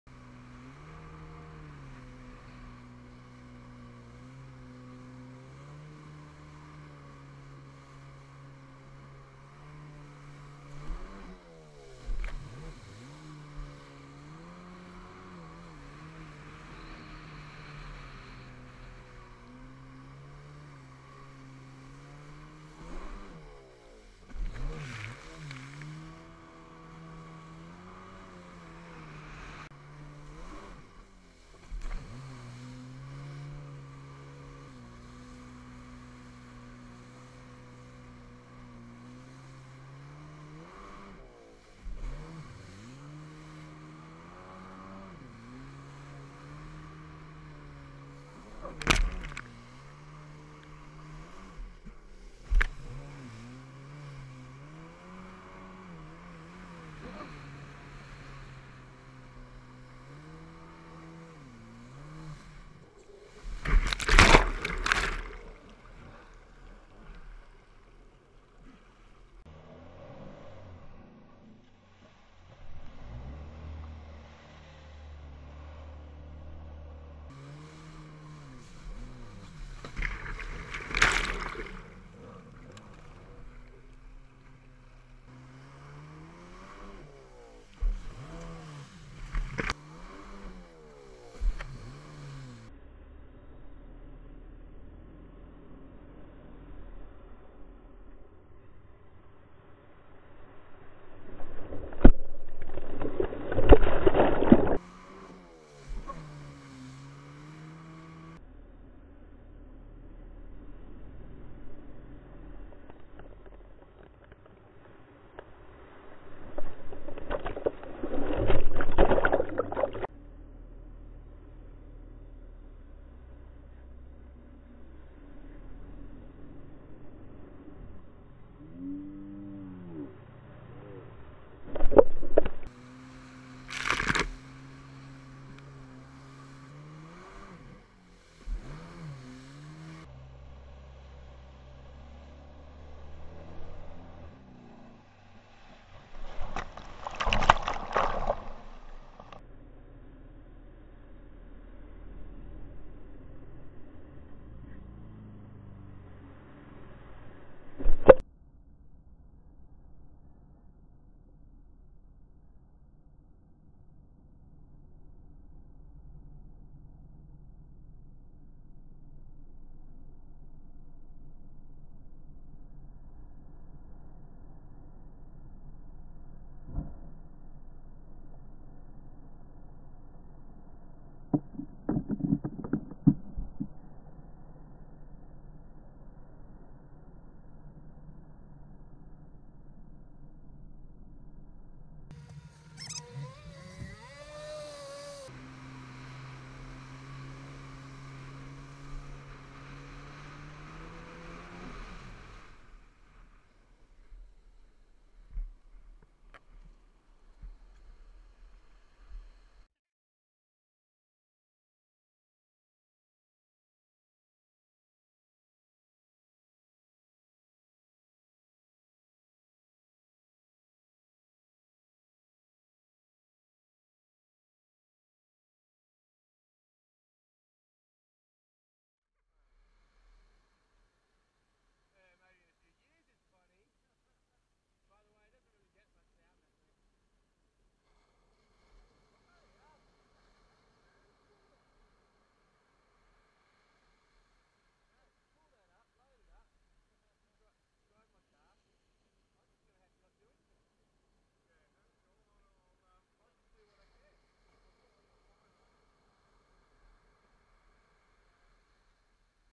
Stand up Jet Ski Jumps, 180 air, broken Achilles tendon Yamaha Superjet
Starts with few jumps, then some attempted 180 airs and ends with Adro snapping his achilles tendon.